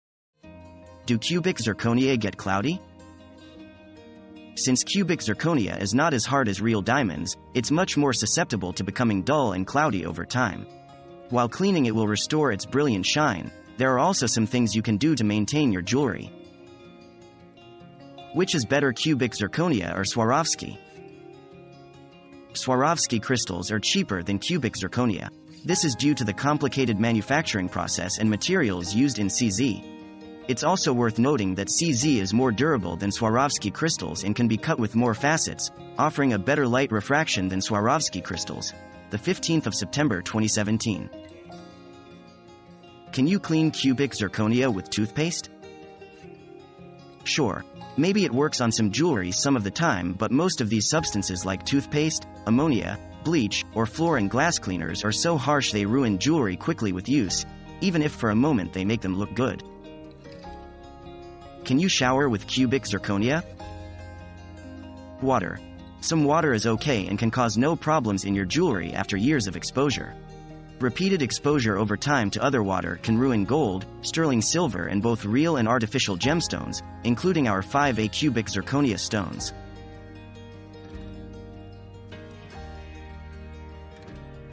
Do cubic zirconia get cloudy?
What Are Man Made Diamonds Called

00:17 - Do cubic zirconia get cloudy?
00:42 - Which is better cubic zirconia or Swarovski?
01:01 - Can you clean cubic zirconia with toothpaste?
01:25 - Can you shower with cubic zirconia?